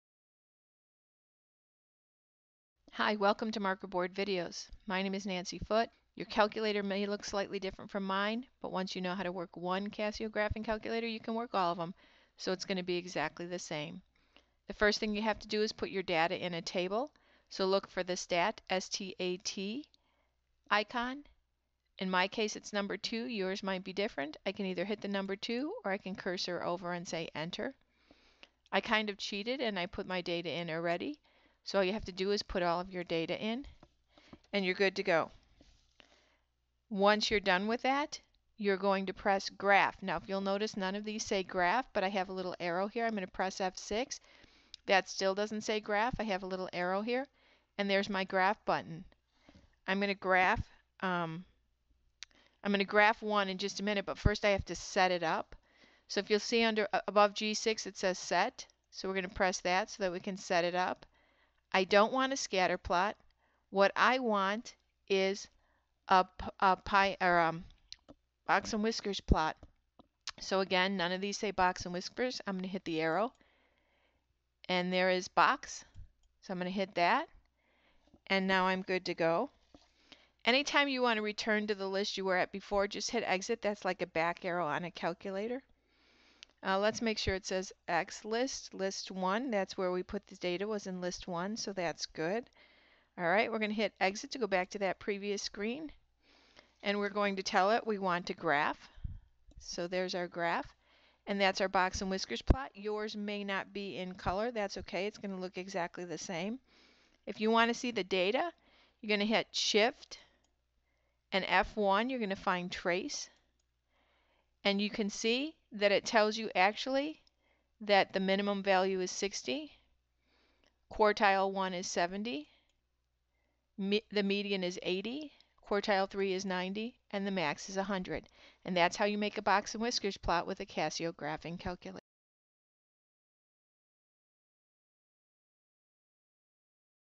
How Do I Make a Box and Whiskers Plot on a Casio Graphing Calculator
In this video you will learn how to make a box and whiskers plot on a Casio graphing calculator.